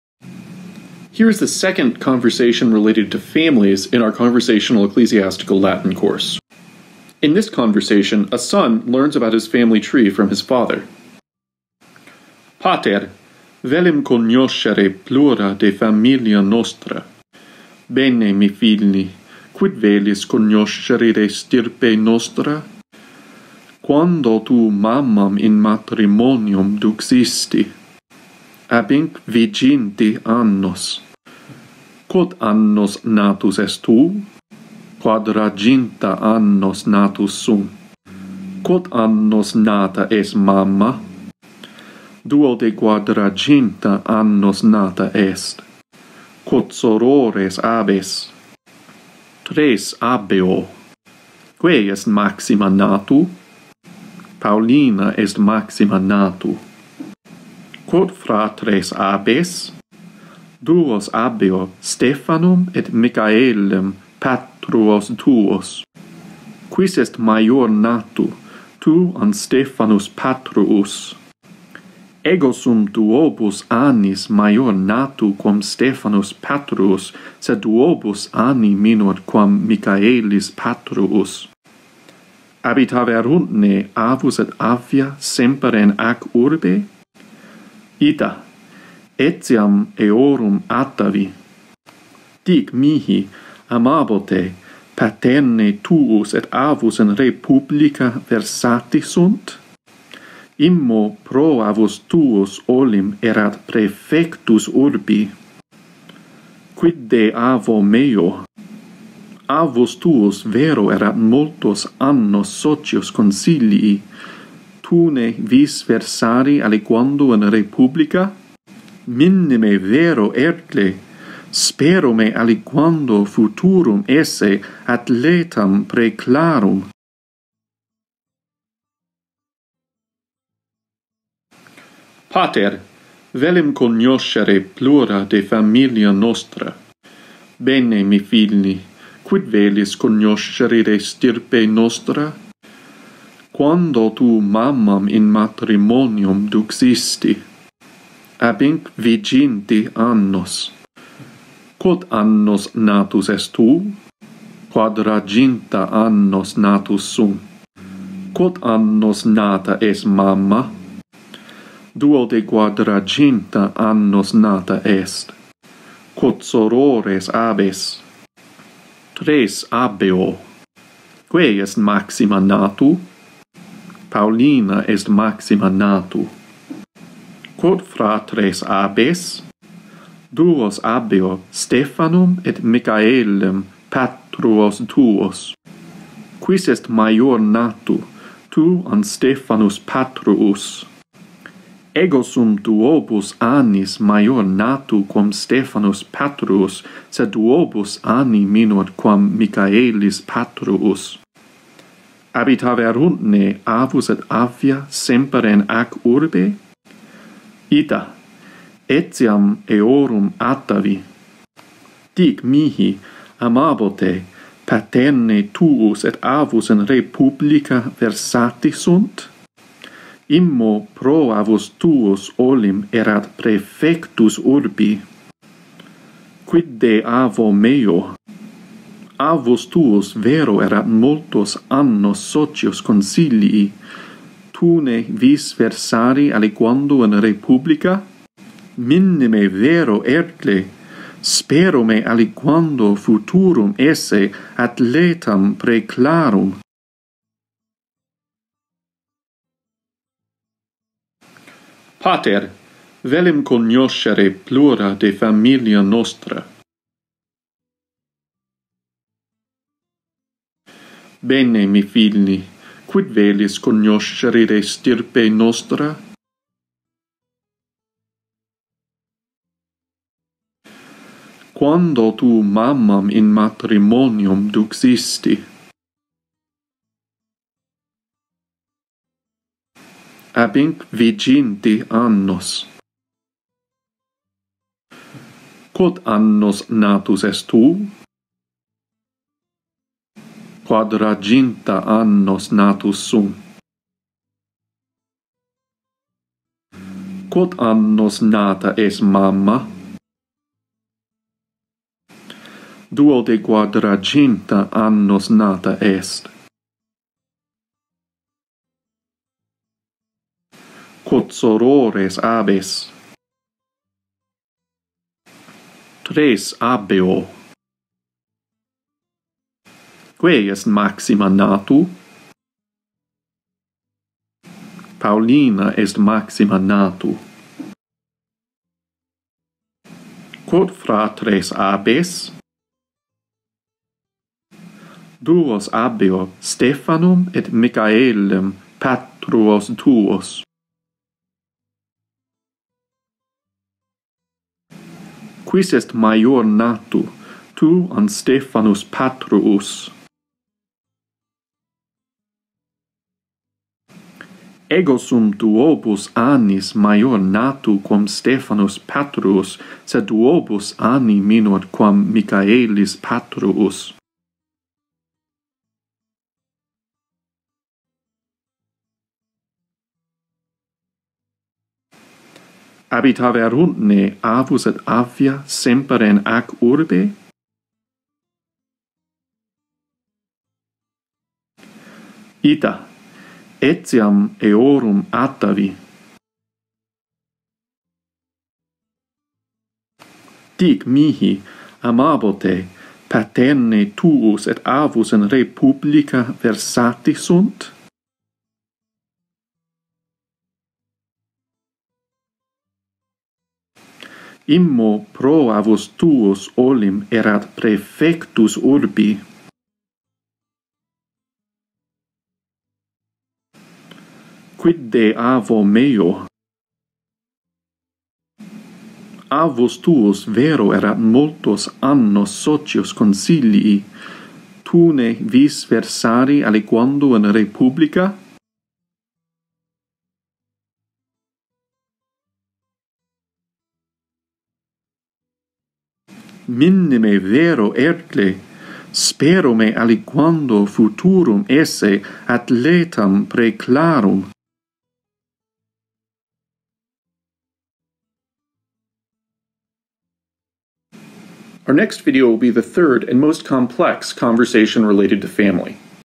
Conversational Ecclesiastical Latin: Family Level 2
In this second family conversation, a son asks his father about their family tree.